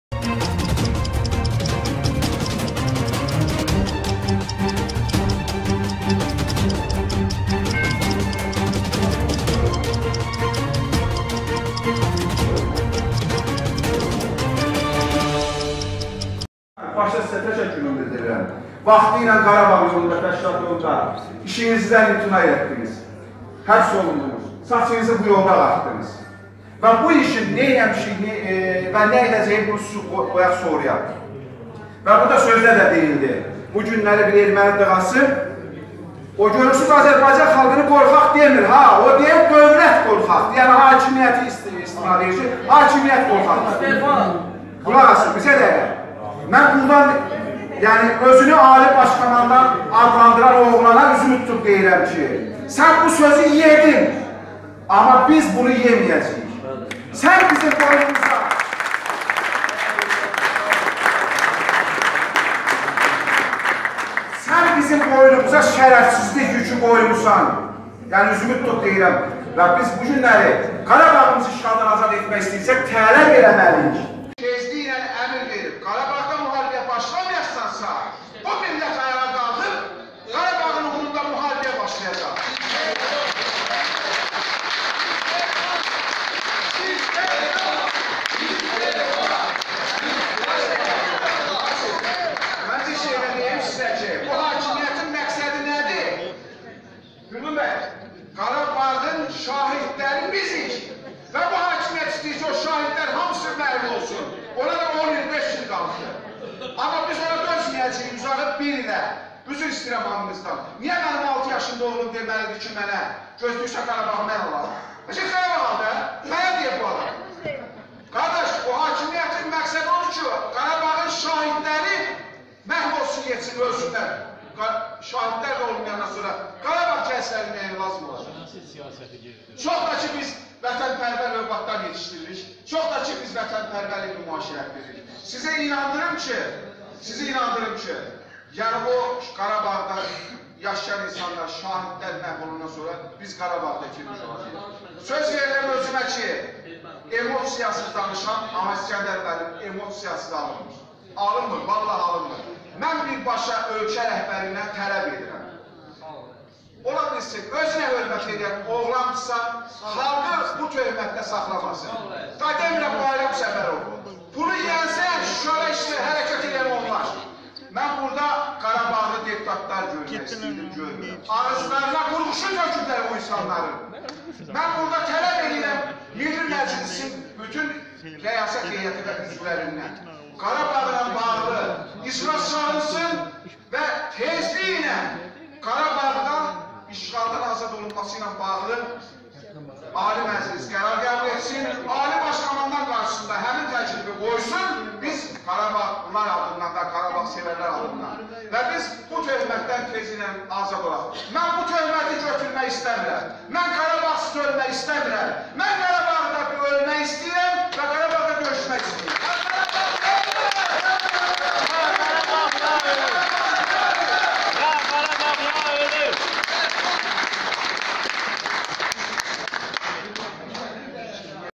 "Üzümü özünü ali baş komandan adlandıran oğlana tutub deyirəm..." -İlqar Əli
Sozcu.az saytı tərəfsiz və qərəzsiz informasiya portalıdır!